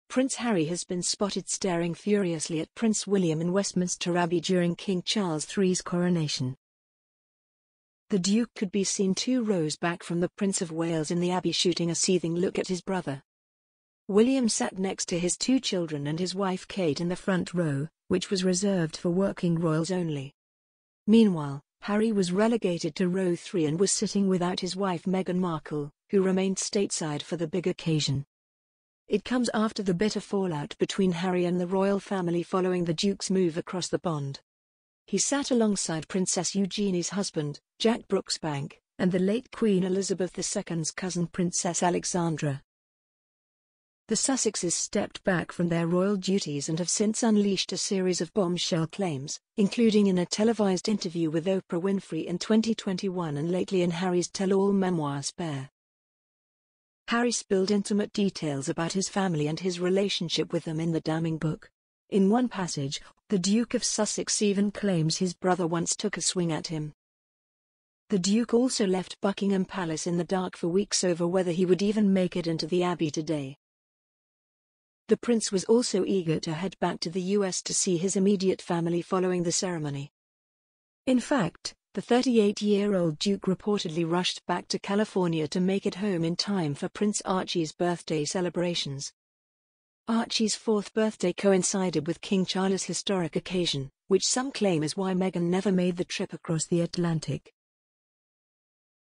Harry's stone-faced stare at William spotted as 'relegated' Duke seethed from third row
Prince Harry has been spotted staring furiously at Prince William in Westminster Abbey during King Charles III's Coronation.

The Duke could be seen two rows back from the Prince of Wales in the Abbey shooting a seething look at his brother.